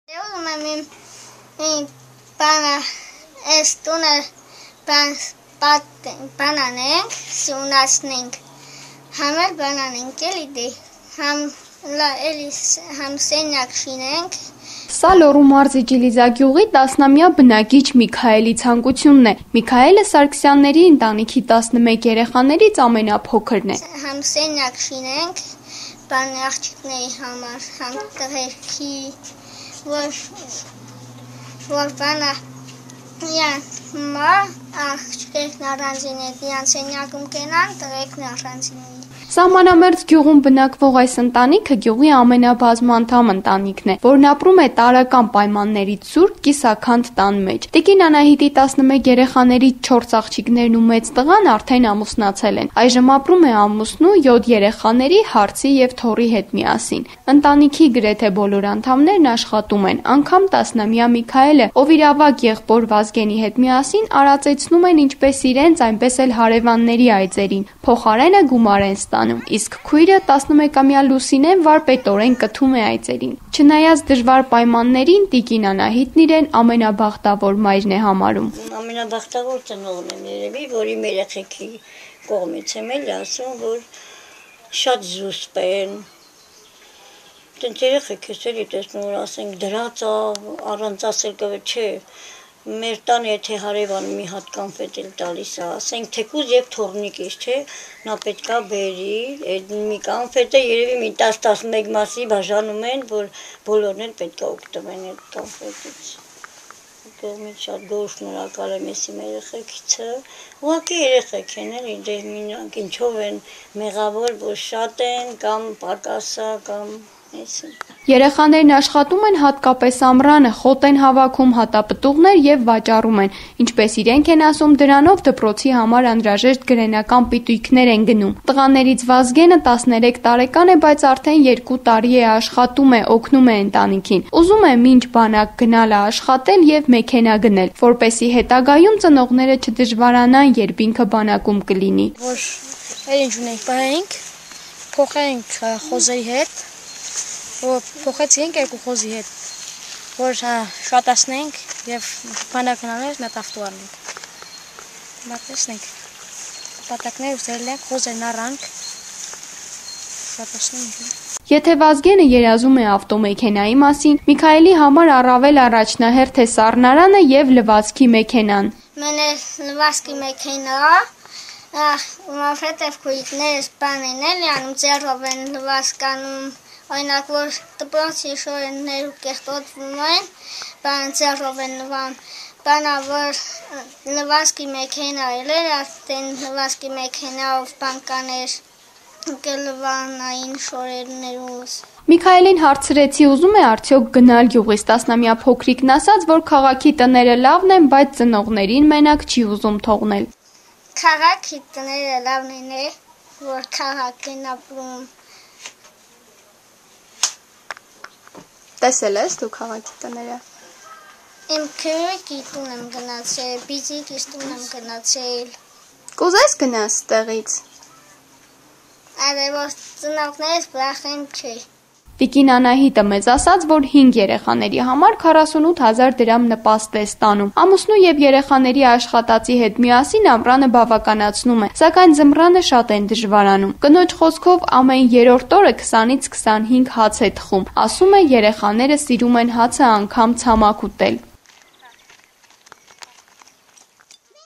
Մերօրյա Գիքորի երազանքը
Լոռու մարզի Ջիլիզա գյուղի 10-ամյա բնակիչ Միքայելն իրենց ընտանիքի 11 երեխաներից ամենափոքրն է: Եթե նրա ավագ եղբայրը երազում է ավտոմեքենայի մասին, ապա ինքը լվացքի մեքենա ունենալու մեծ ցանկություն ունի, որպեսզի քույրերը ձեռքով լվացք չանեն: Բայց ամենամեծ երազանքն ընդարձակ տուն ունենալն է, որտեղ տղաների ու աղջիկների սենյակներն առանձնացված կլինեն: